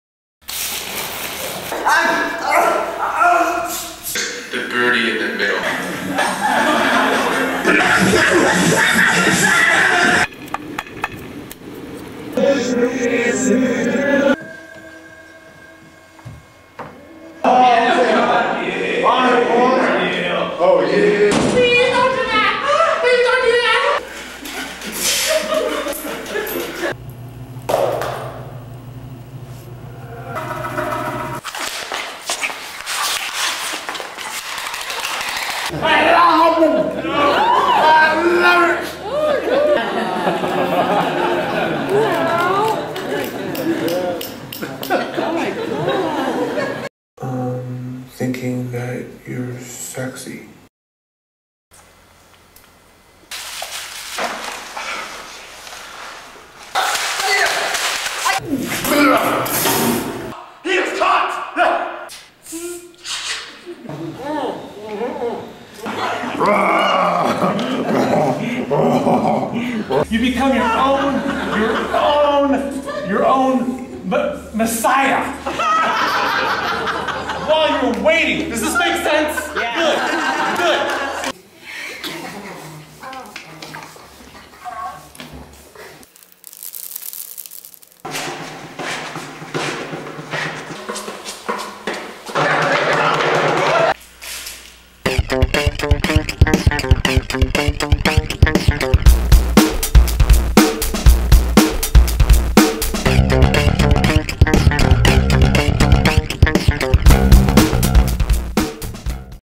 Scotch Wichmann - Performance Art
SCOTCH WICHMANN joined L.A.'s underground performance art scene at age 19 with his debut pieces, SNORTING MOUSE FUR and HAVING A BALL: ONE TESTICLE'S PUPPET SHOW. His performance work is regularly featured at galleries and fringe festivals around the world, where his pieces have become known for their magical realism, physical endurance, and Dadaist pranks.

His debut novel, TWO PERFORMANCE ARTISTS KIDNAP THEIR BOSS AND DO THINGS WITH HIM, goes on sale in April, 2014.